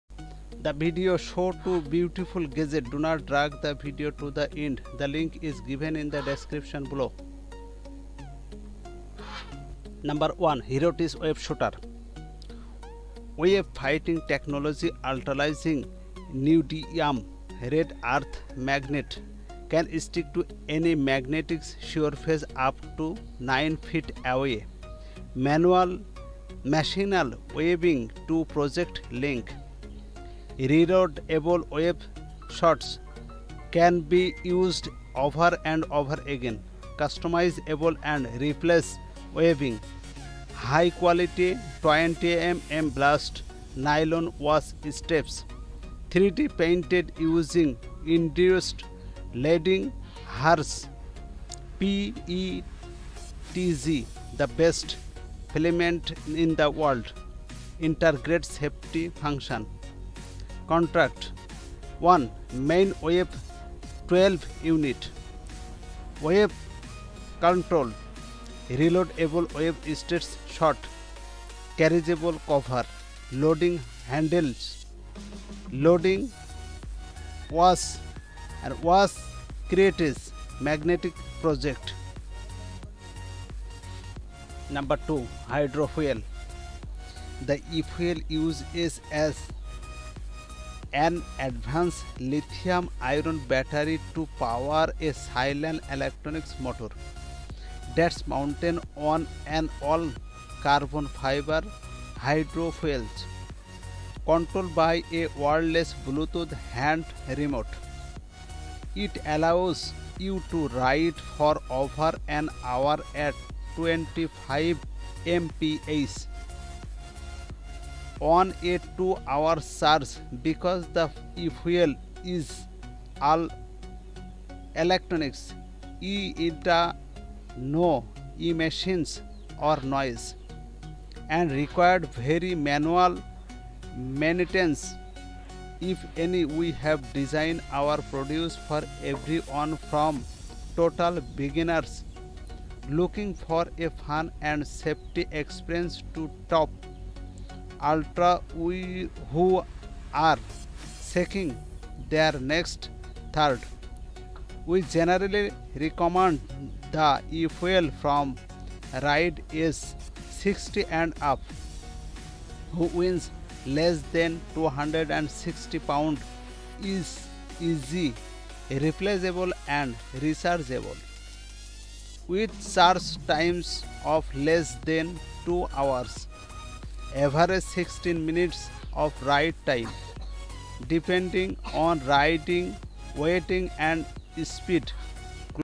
Herotech web shooter review | Herotech web shooter buy.
This video shows HEROTECH WEB SHOOTER and HYDROFOIL. The first product to be shown is the HEROTECH WEB SHOOTER watch. Product number two is shown to HYDROFOIL. This HYDROFOIL can move easily on water. Battery powered and with remote control.
                       Gadgets buying Links:
                                       1.HEROTECH WEB SHOOTER

                          Necessary gadgets for your everyday life
                        Which gadgets did you like?Write in the comments.